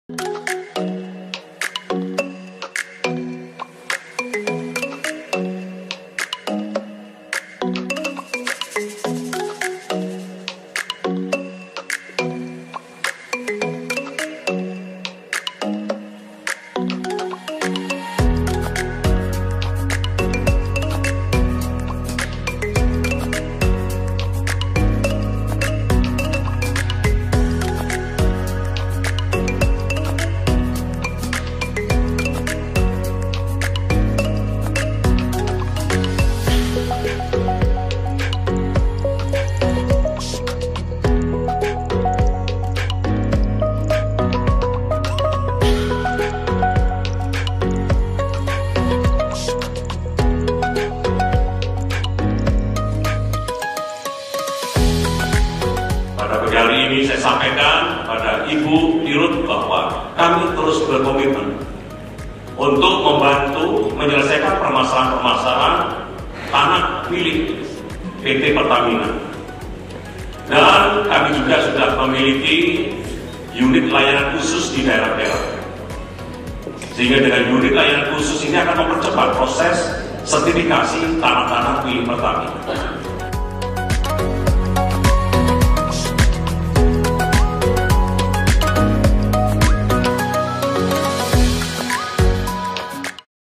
PENYERAHAN SERTIPIKAT PT PERTAMINA PERSERO
Halo SobATRBPN, simak bersama yuk!
Kegiatan Menteri ATR/Kepala BPN, Hadi Tjahjanto bersama Wakil Menteri ATR/Wakil Kepala BPN, Raja Juli Antoni saat melakukan penyerahan Sertipikat Tanah Proyek Strategis Nasional PT Pertamina (Persero).

Nah, dalam kesempatan ini Menteri ATR/Kepala BPN menyampaikan komitmen Kementerian ATR/BPN untuk terus menyelesaikan permasalahan aset pertanahan PT Pertamina (Persero).